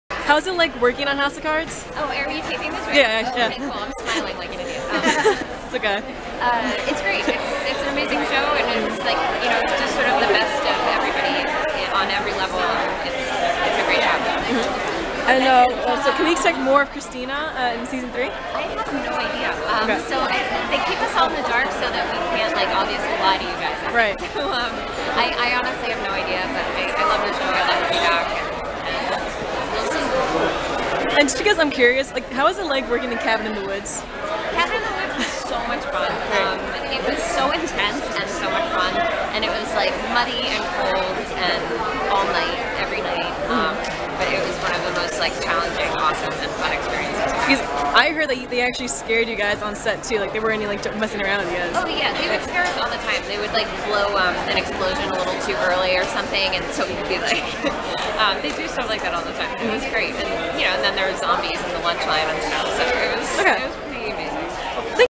Kristen Connolly talks House of Cards and Cabin In The Woods
At the 2014 Tribeca Film Festival